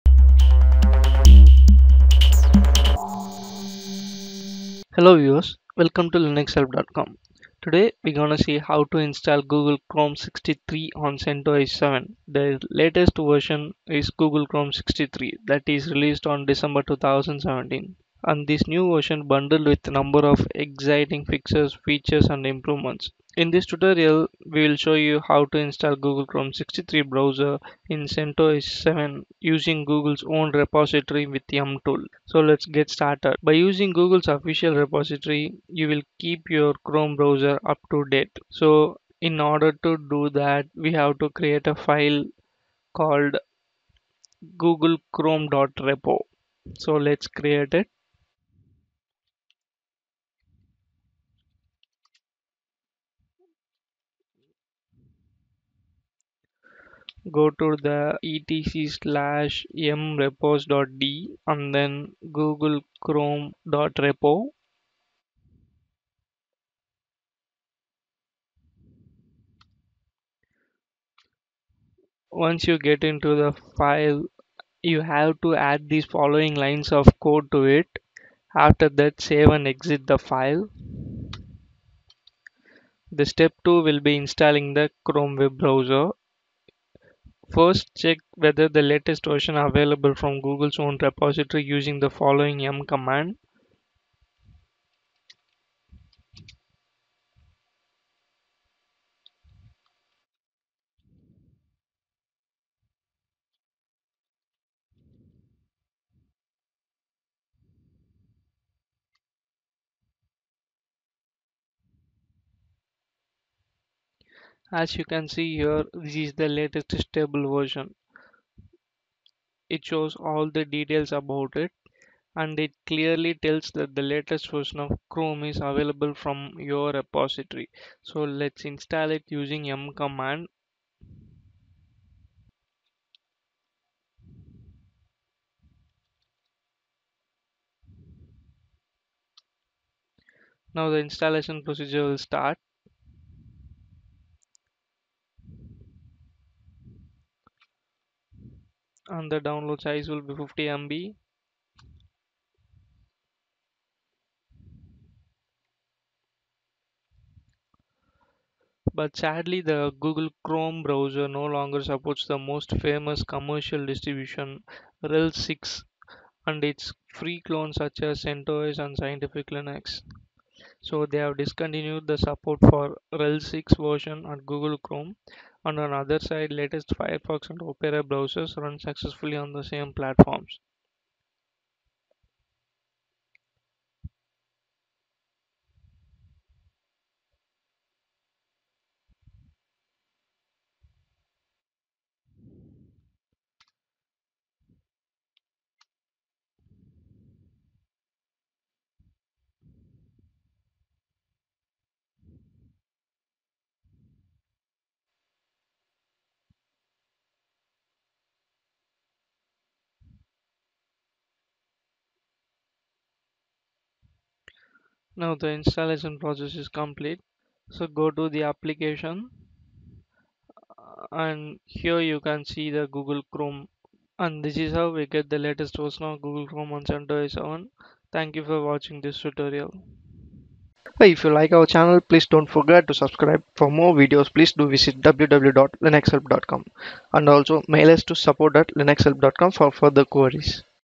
How to Install Google Chrome 63 on CentOS 7
This video covers the method to install Google Chrome 63 on CentOS 7. Google Chrome is a freeware web browser developed by Google Inc.